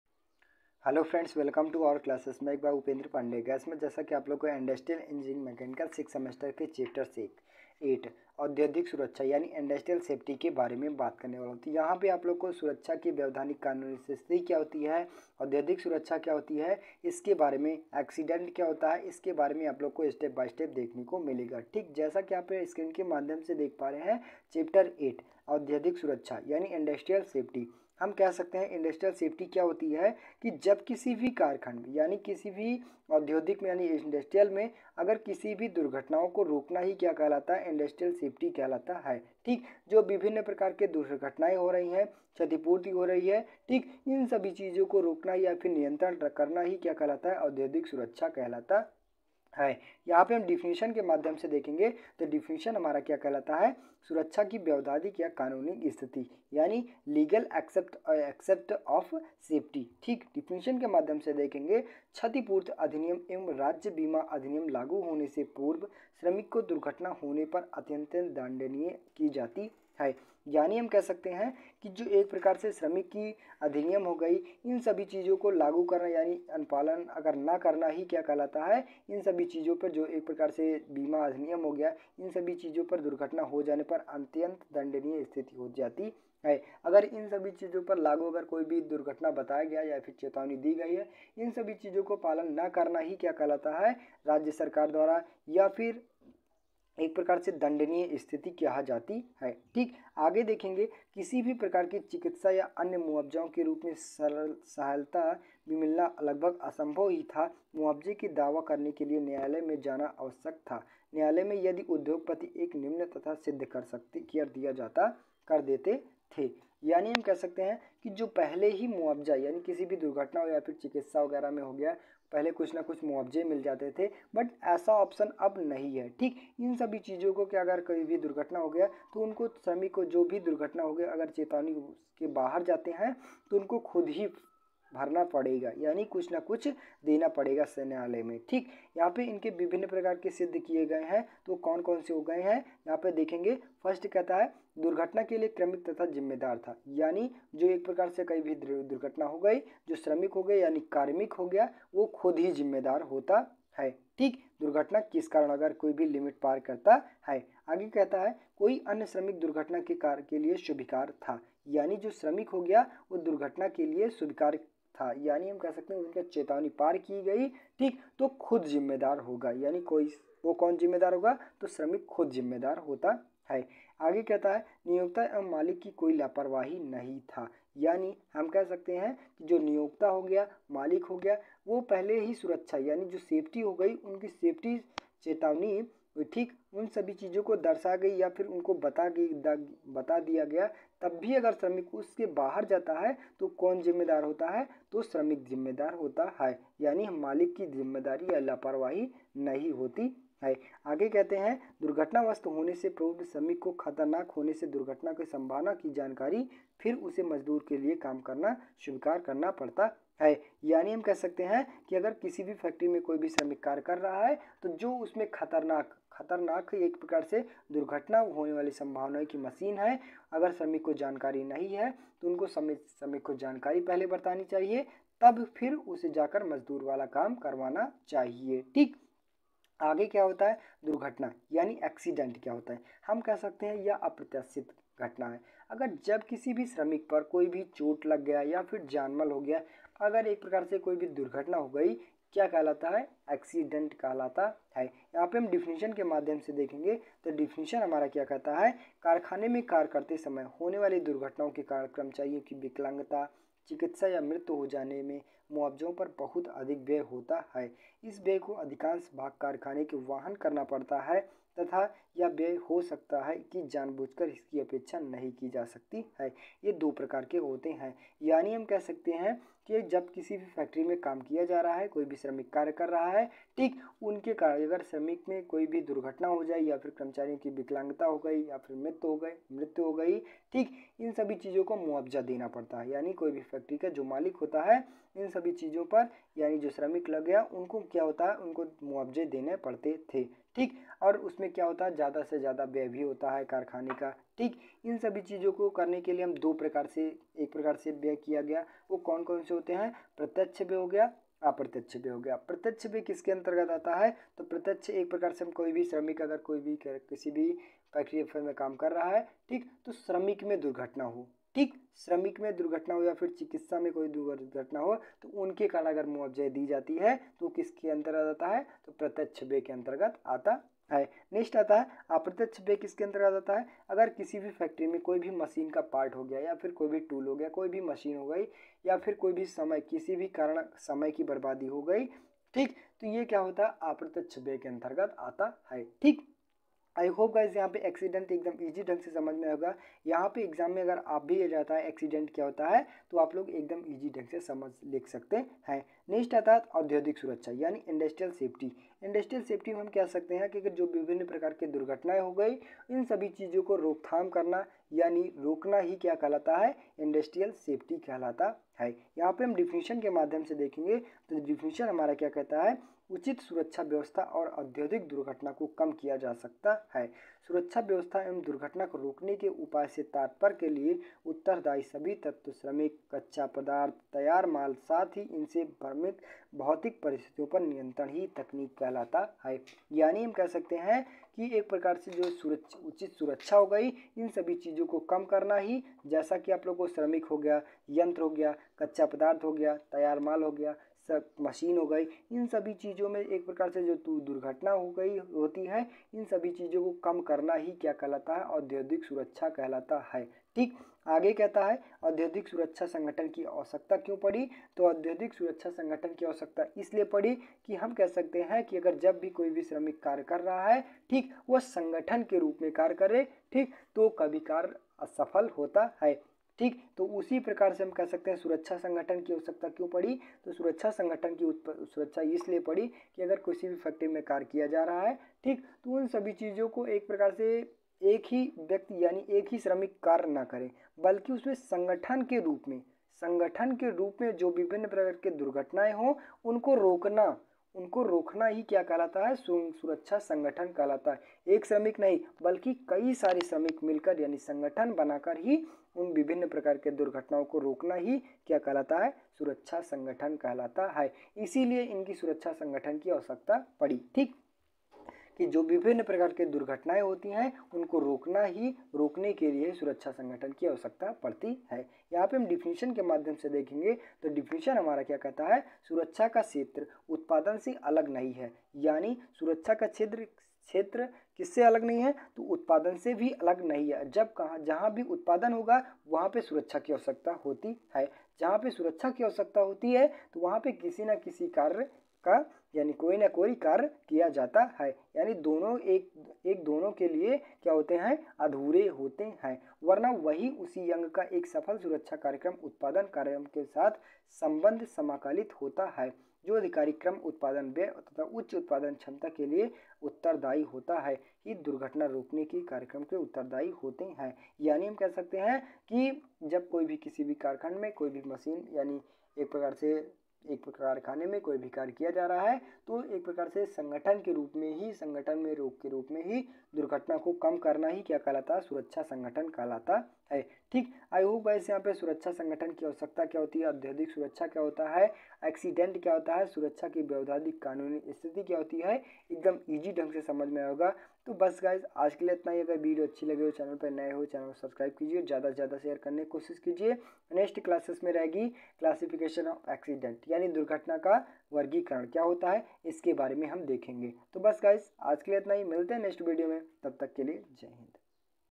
Machanical 6th semester / Industrial Engineering / chepter-8 / Industrial Safety / lecture 22 /
video topic 👇👇👇👇

legal aspects of safety
accident
industrial Safety
need for safety organization